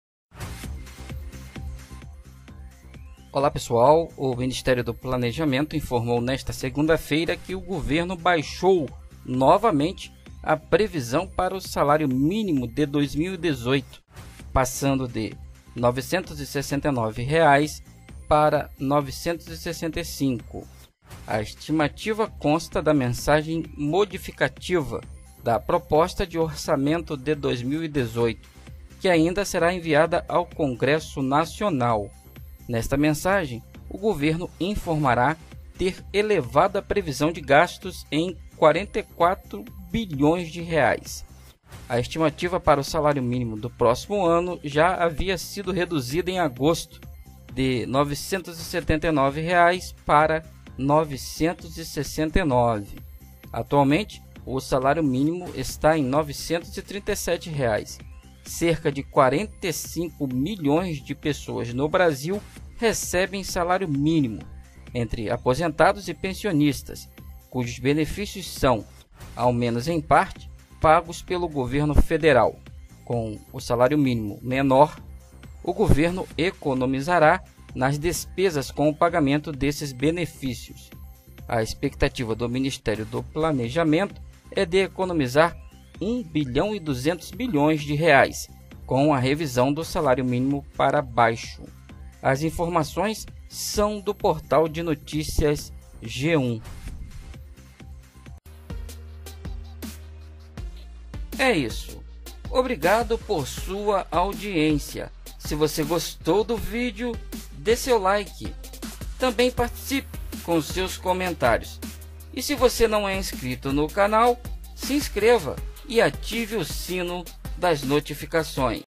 Governo reduz previsão para o salário mínimo de R$969 para R$965.
Anúncio foi feito nesta segunda pelo Ministério do Planejamento. Governo informou que redução se deve à previsão menor para a inflação de 2017, que serve de base para reajuste do mínimo. Fonte: G1